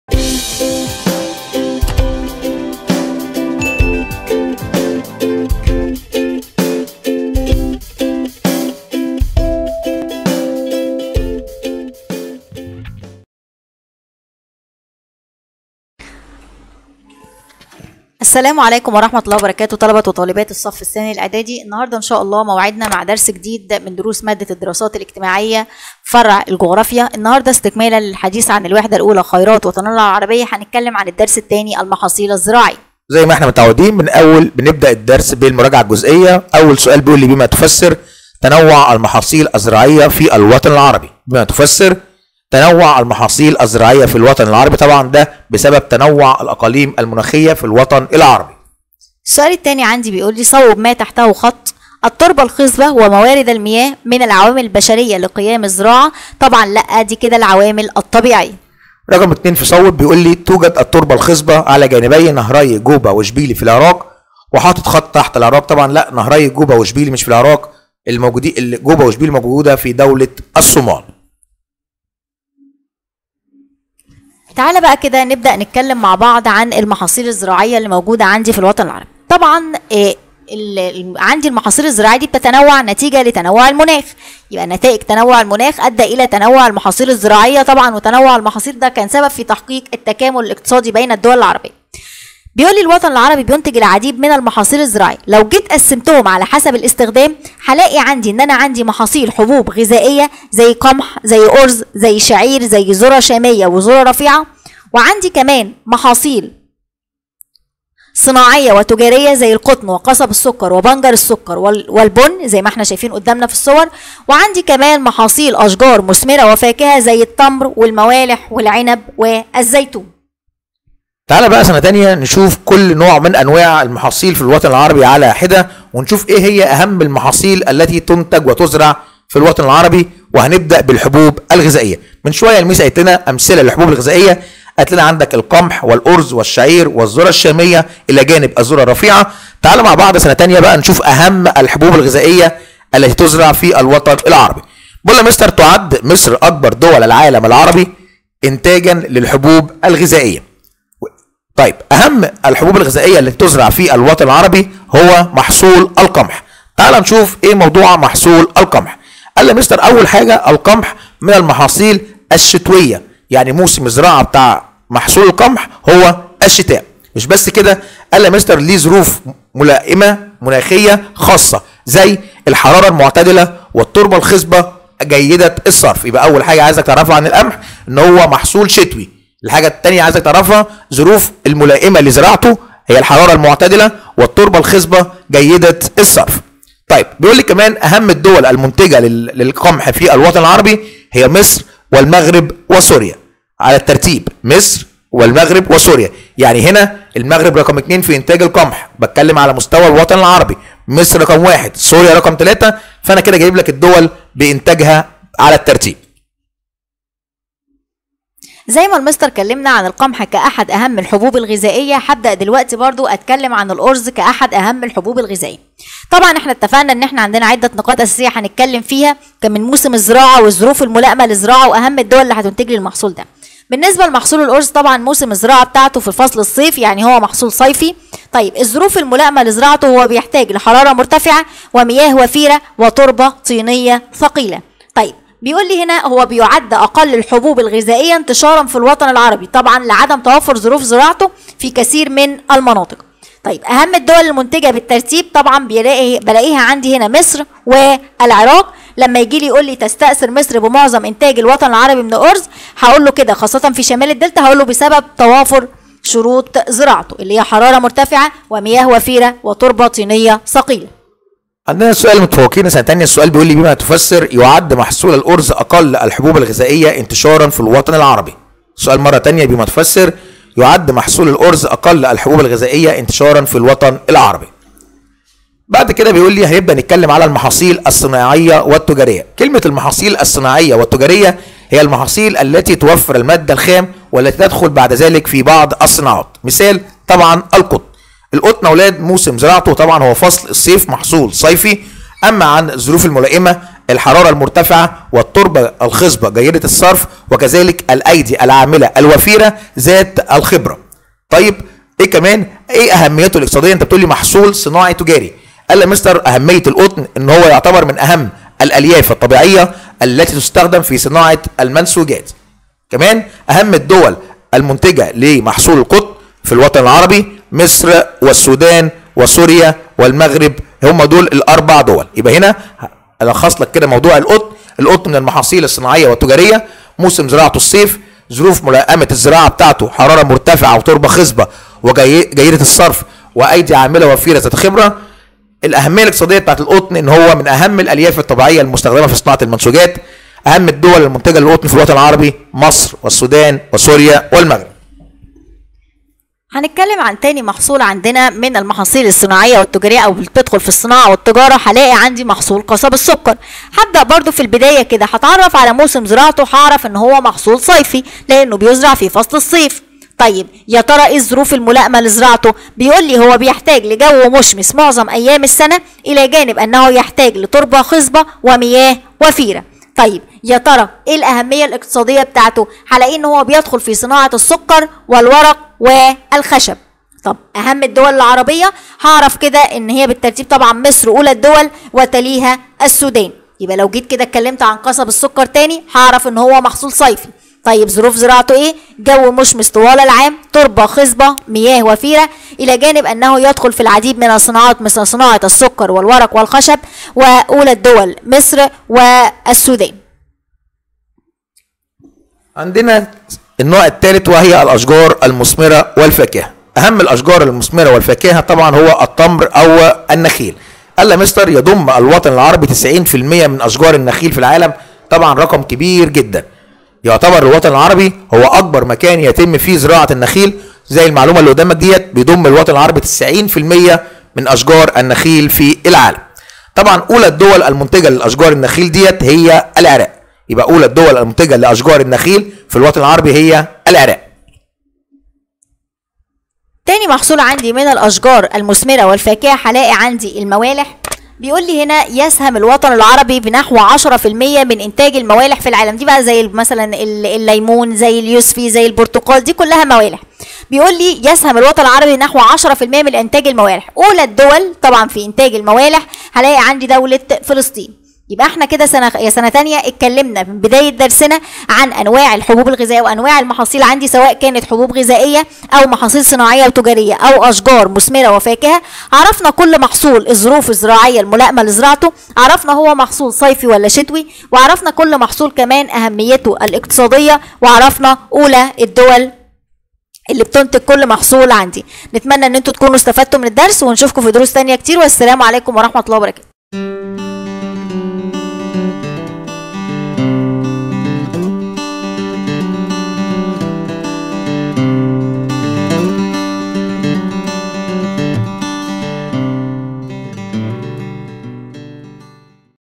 المحاصيل الزراعية في الوطن العربي | الدراسات الاجتماعية | ثانية إعدادي | قناة حراء التعليمية
المحاصيل الزراعية في الوطن العربي

شاهد .. ناقش .. إحجز مكانًا في المستقبل